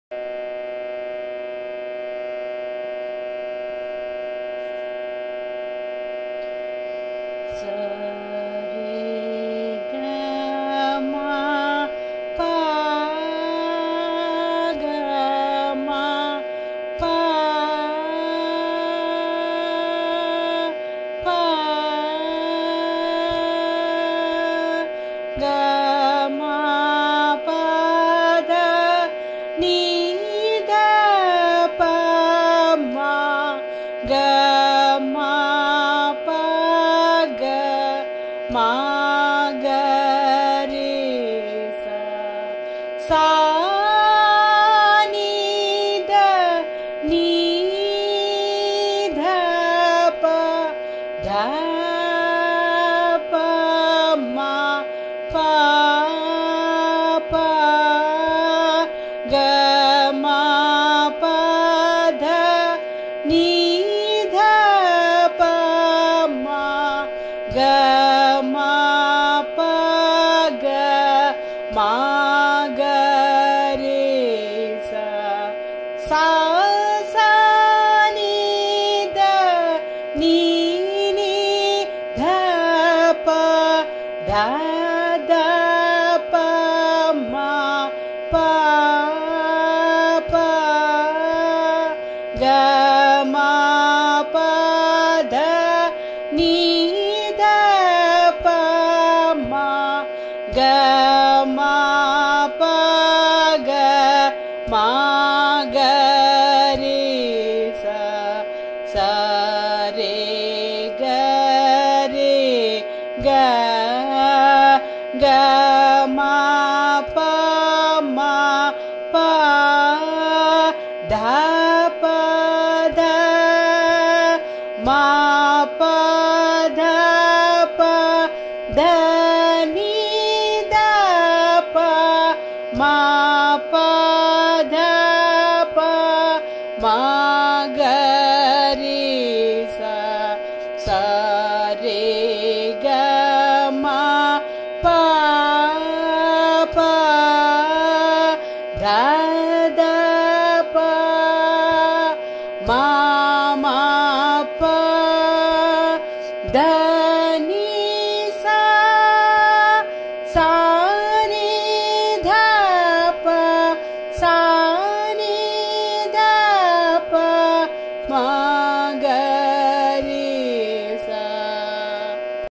Beginners' Lesson MADHYA STHAYI VARISAI
Madhya Sthayi Varisai
1. || s r g m p , g m | p , , , | p , , , || g m p d n d p m | g m p g | m g r s ||
2. || S , n d n , d p | d , p m | p , p , || g m p d n d p m | g m p g | m g r s ||
3. || S S n d n n d p | d d p m | p , p , || g m p d n d p m | g m p g | m g r s ||
4. || s r g r g , g m | p m p , | d p d , || m p d p d n d p | m p d p | m g r s ||
5. || s r g m p , p , | d d p , | m m p , || d n S , S n d p | S n d p | m g r s ||
The purpose of sarali varisai is to facilitate singing in different speeds. It helps to provide
with necessary rhythmic and melodic foundation.